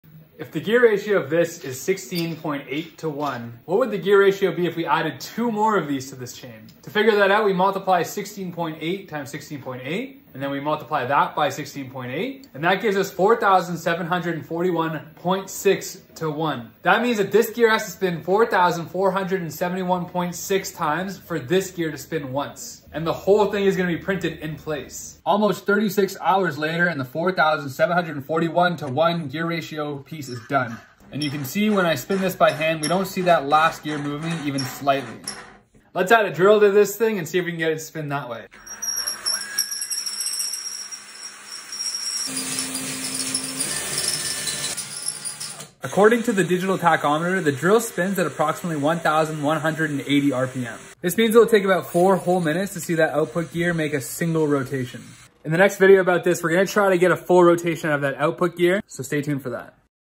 4741:1 Gear Ratio vs Drill!
Massive Gear Ratio vs Drill
The gear ratio is 4741 to 1, which means it will take 4741 rotations of the input gear to get one single rotation at the output. In other words, it will take 4 full minutes of drill powered rotation (at full speed) to get the single output rotation. •••